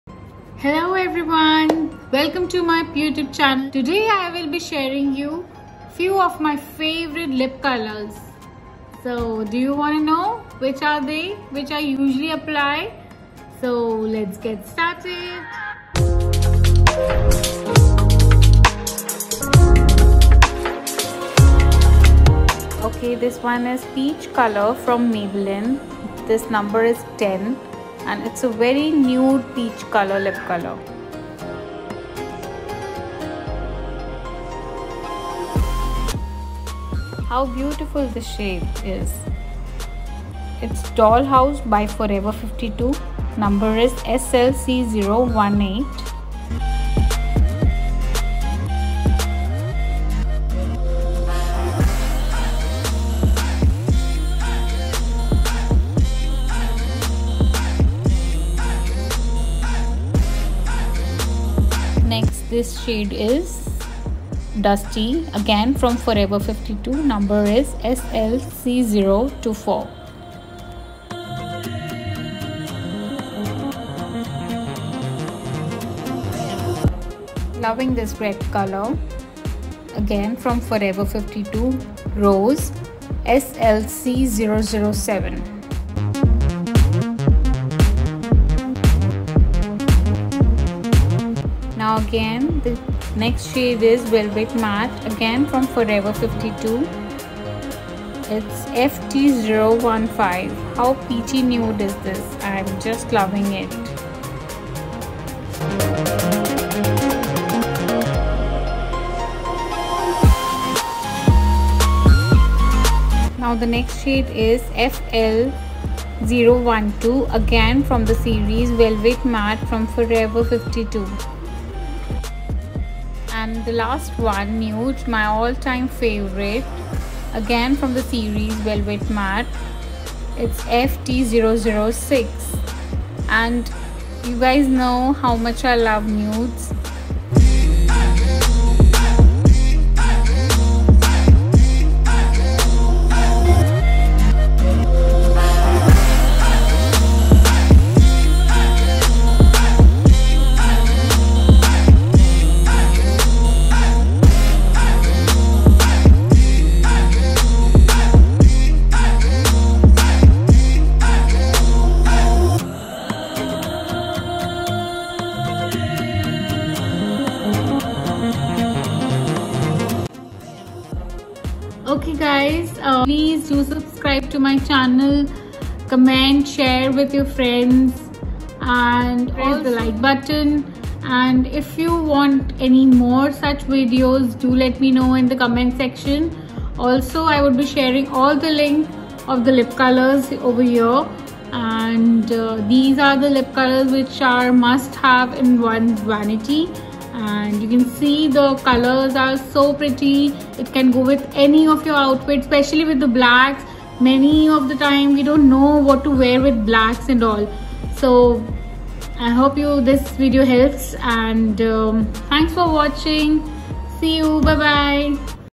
Lip swatches from my favourite brands || Glam factory by komal
Hi
Today’s tutorial is all about lipstick my favourite  lip colour swatches. I promise you would love it.
Do share your reviews in the comment section……

Lip colour : 1 - @maybeline
Colour sensational 10
2 : @forever52 Doll house SLC018
3: @forever52 Dusty SLCO24
4: @forever52 Rose SLC007
5: @forever52 velvet matte FT015
6: @forever52 velvet matte FL012
7: @forever52 velvet matte FT006

*Disclamer*